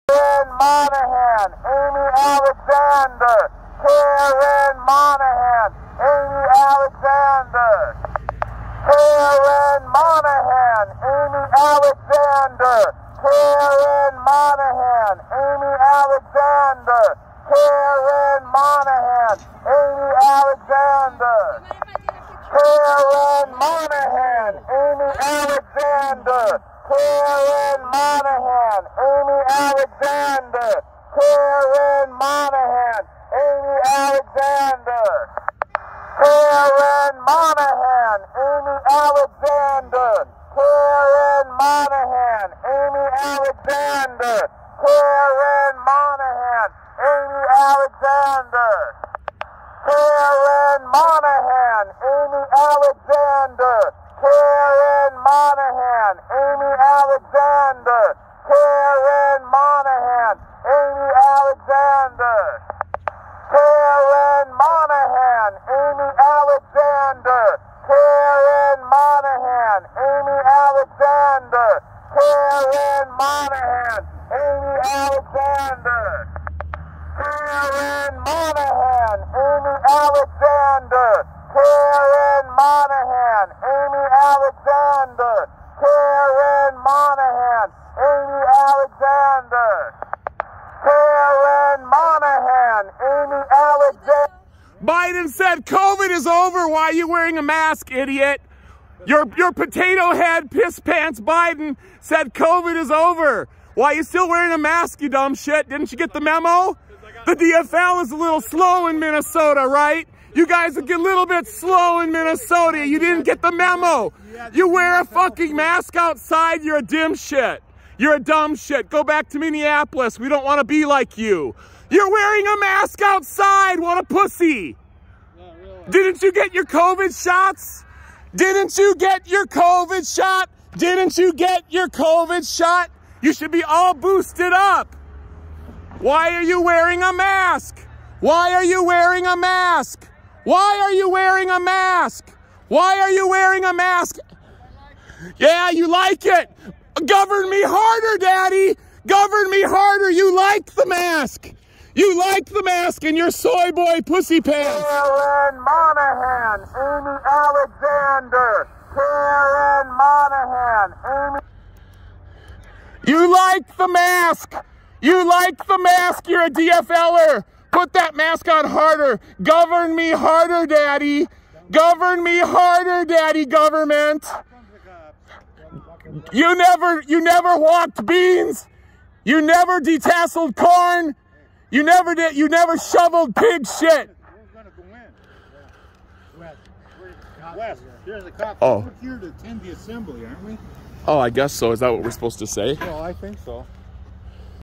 Keith Ellison (AKA MUHAMAD HAKIM)'S Henchmen Hear "Karen Monihan Amy Alexander" in Rochester, MN.
Keith Ellison (AKA Muhammad Hakim) Sends Henchmen to Document Opposition to his event.  My chant is simple: "Karen Monihan Amy Alexander".